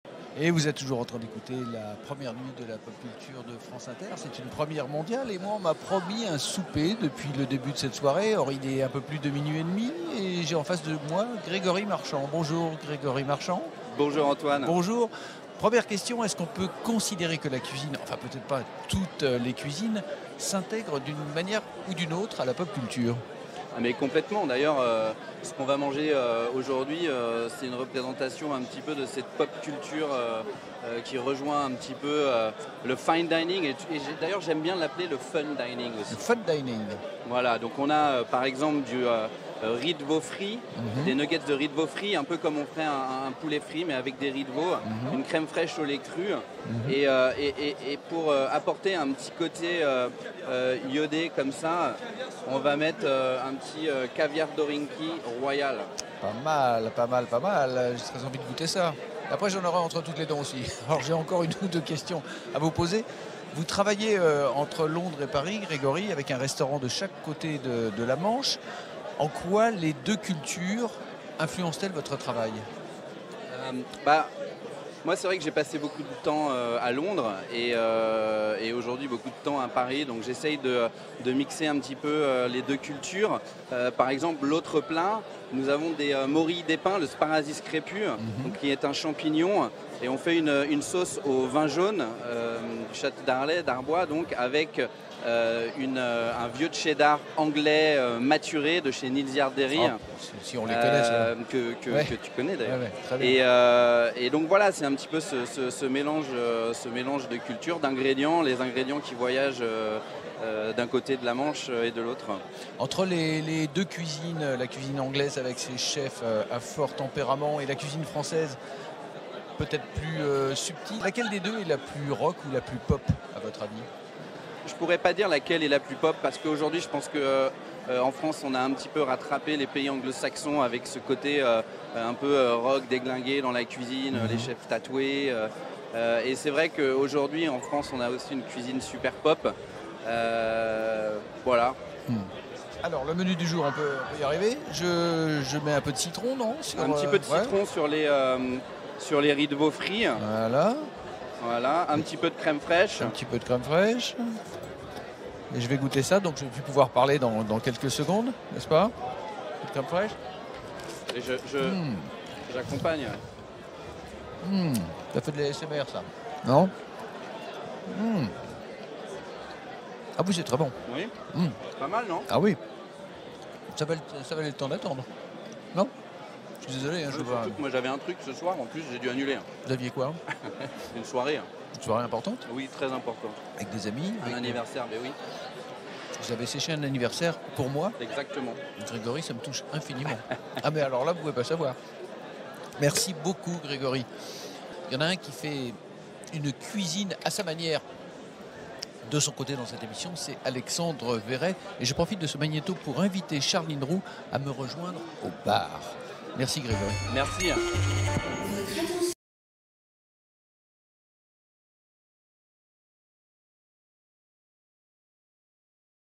Gregory Marchand : "La cuisine s’intègre complètement à la pop culture"
Est ce que la cuisine s’intègre à la pop Culture demandait un Antoine de Caunes gourmand au chef étoilé du Frenchie restaurant Gregory Marchand.
Pour cette nuit de la pop culture il raconte sa vision de la cuisine, les influences de la cuisine britannique et française sur son travail.